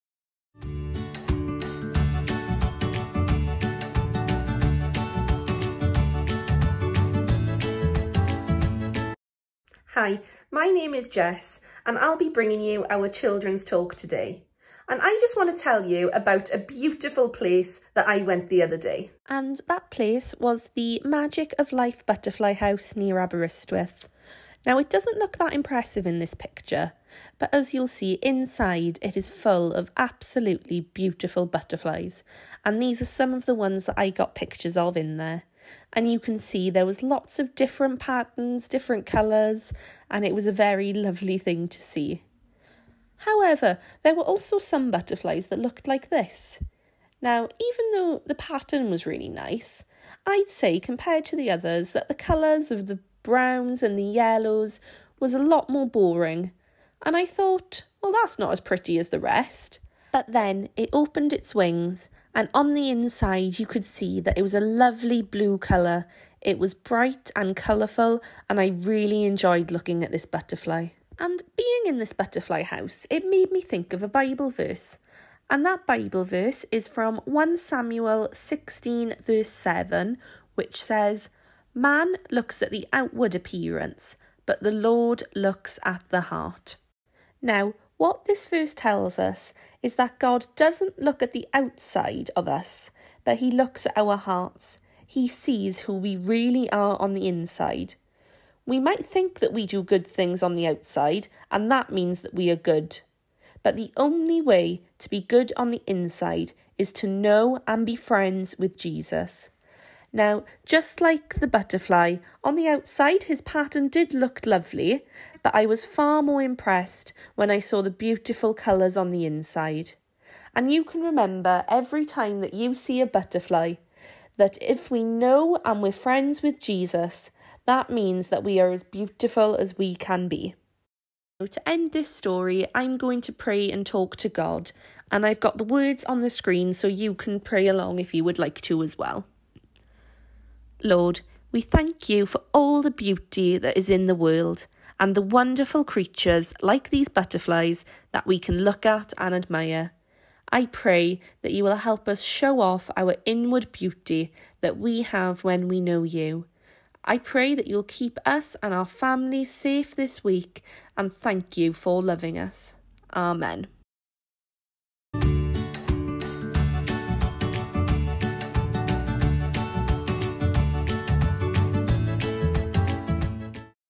Kids talk (Butterflies)
Our Latest video from Libanus Church, Wales. We hope you will be blessed as you learn more about Jesus and his work on the cross.

CCLI: 745089

Always happy to answer any questions, God bless.